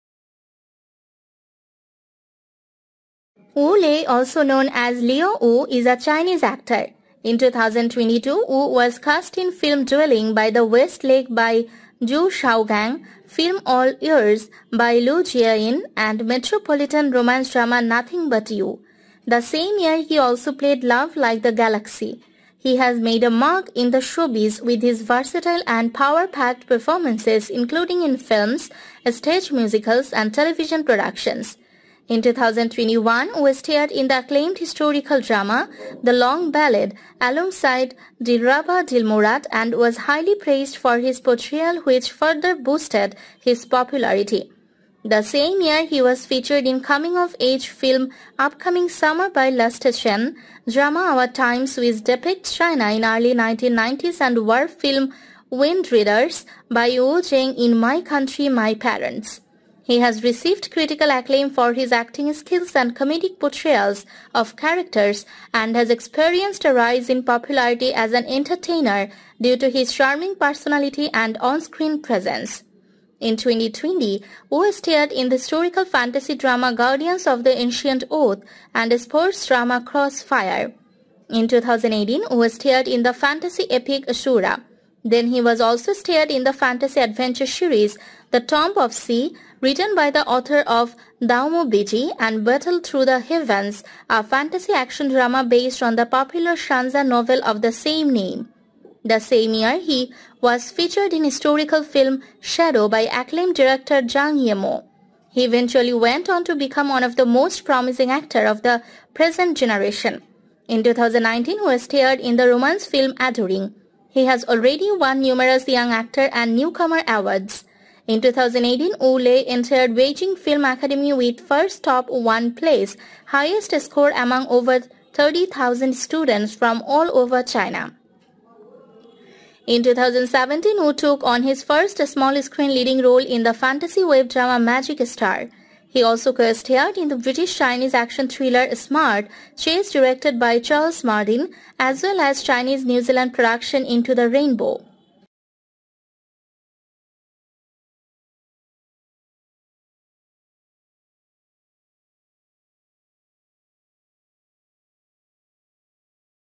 Leo Wu (Love Like the Galaxy) || Top 12 Interesting Facts About Leo Wu
Leo Wu || Top 12 Interesting Facts About Leo Wu

Wu Lei, also known as Leo Wu, is a Chinese actor.
In 2022, Wu was cast in film Dwelling by the West Lake by Gu Xiaogang, film All Ears by Liu Jiayin and metropolitan romance drama Nothing but You.
The same year he also played Love Like the Galaxy.
He has made a mark in the show biz with his versatile and power packed performances including in films, stage musicals and television productions.
In 2021, Wu starred in the acclaimed historical drama, The Long Ballad, alongside Dilraba Dilmurat and was highly praised for his portrayal, which further boosted his popularity.